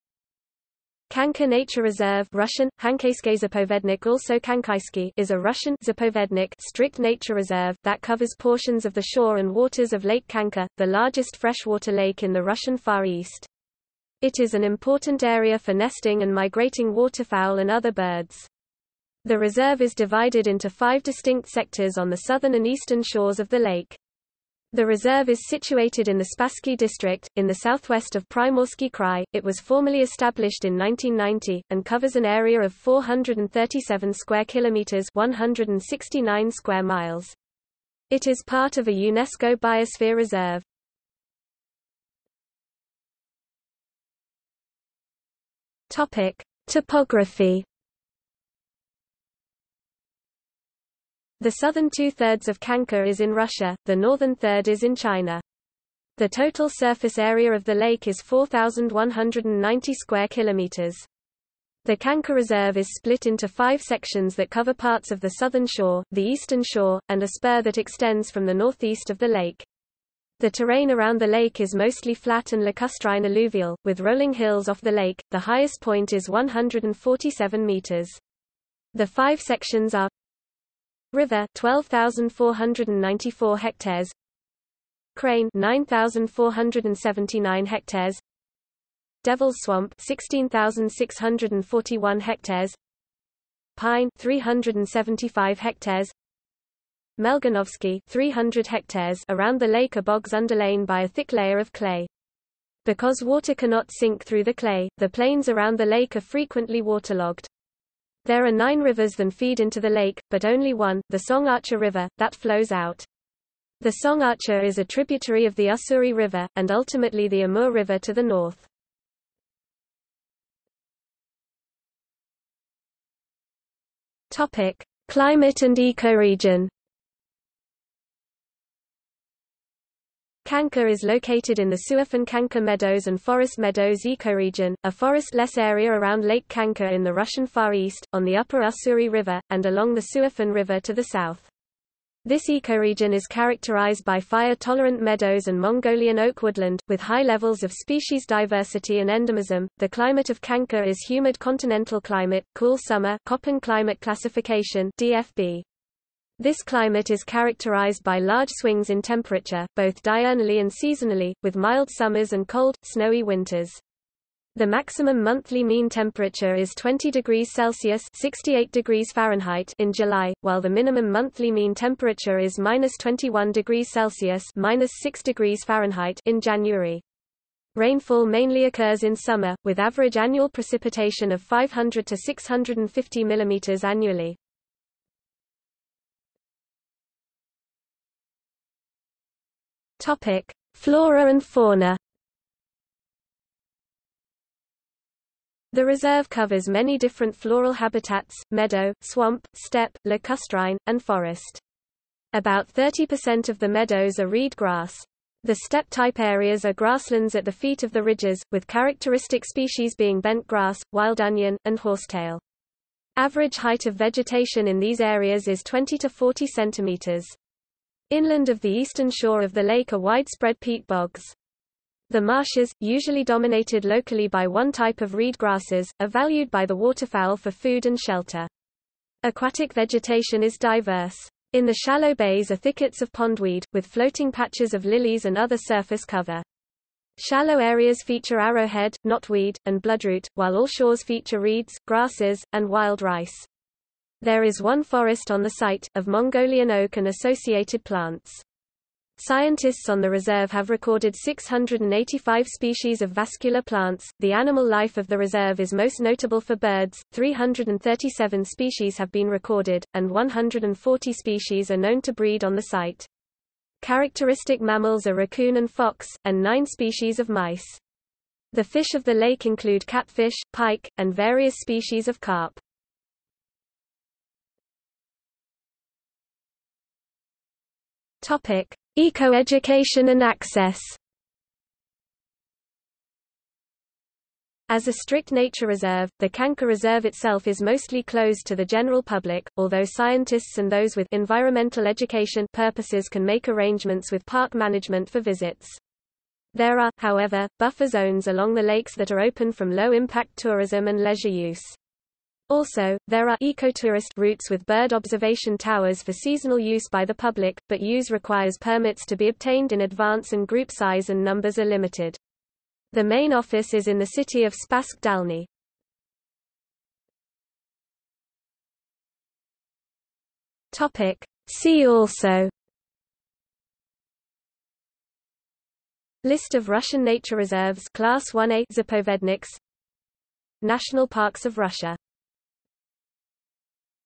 Khanka Nature Reserve | Wikipedia audio article
00:00:42 1 Topography
00:02:00 2 Climate and Ecoregion
00:03:06 3 Flora and fauna
00:04:34 4 Ecoeducation and access
00:05:17 5 See also

- Socrates

SUMMARY
Khanka Nature Reserve (Russian: Ханкайский заповедник) (also Khankaisky) is a Russian 'zapovednik' (strict nature reserve) that covers portions of the shore and waters of Lake Khanka, the largest freshwater lake in the Russian Far East.  It is an important area for nesting and migrating waterfowl and other birds.  The reserve is divided into five distinct sectors on the southern and eastern shores of the lake.  The reserve is situated in the Spassky District, in the southwest of Primorsky Krai.  It was formally established in 1990, and covers an area of 437 km2 (169 sq mi).  It is part of a UNESCO Biosphere Reserve.